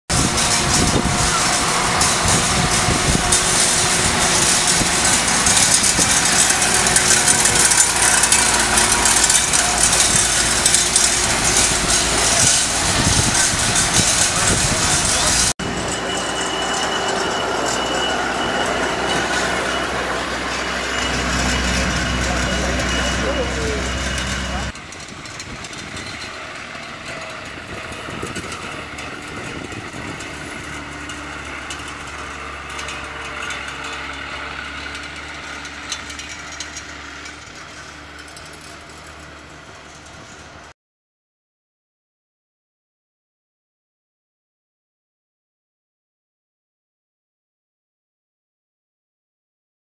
Citroen Kegresse autochenille on the move
Seen in Meudon, during the classic car gathering called "Traversée de Paris", where hundreds of classic cars drive across the capital of France.